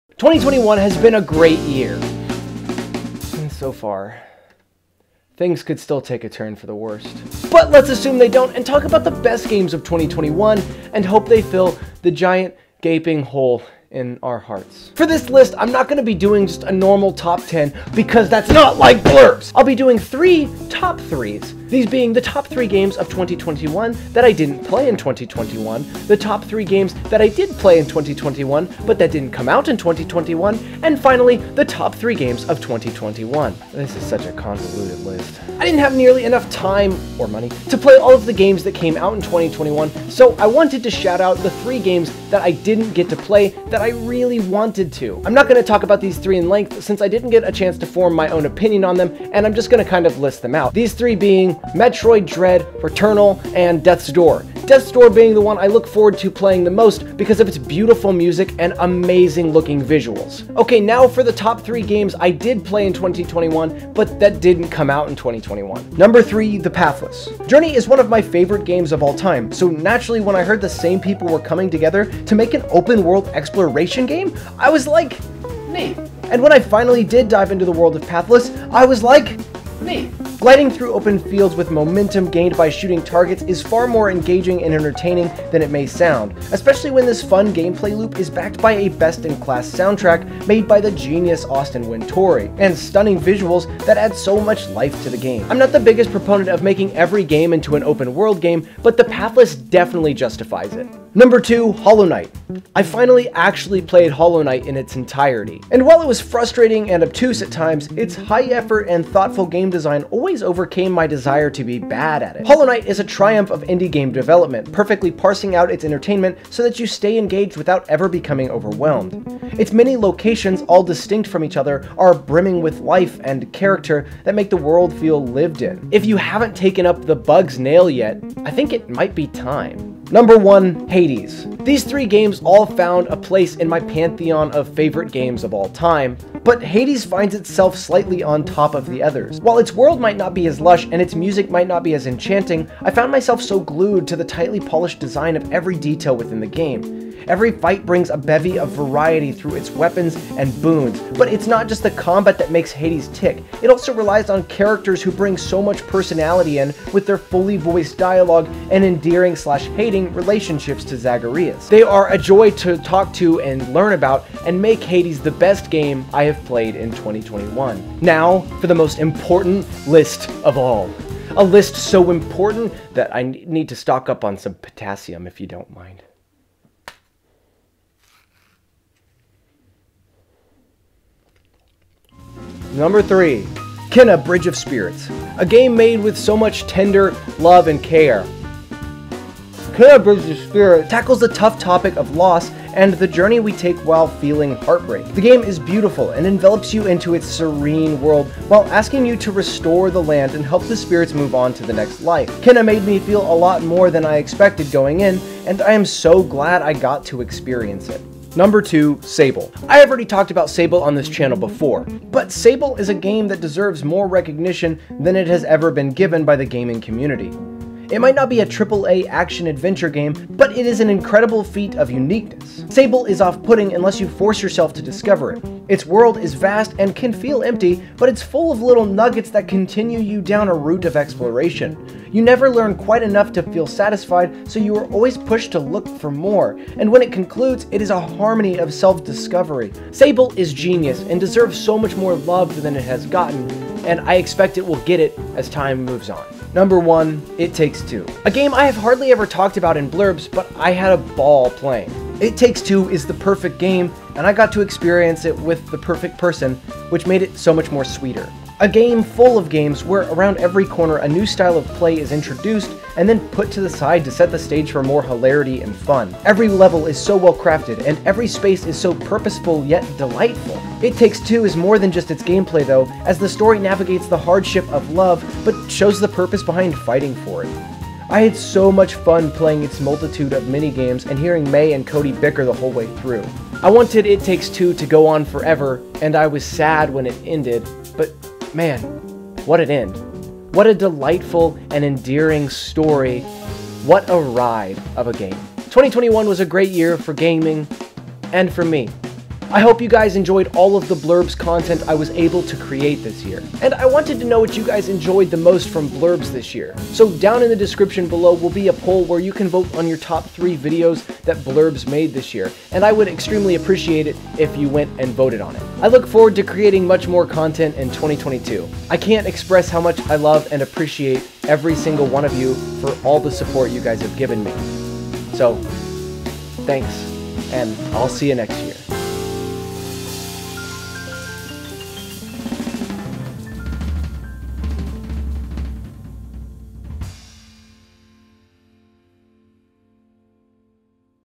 My Favorite Games of 2021
Hope you enjoyed my video, feel free to vehemently disagree with me in the comment section below.

Contents:
0:00 - Intro
0:35 - Top 3 Games I Didn't Play
1:00 - Top 3 Games I Played in 2021, But Not From 2021
2:48 - Top 3 Games of 2021
5:00 - Outro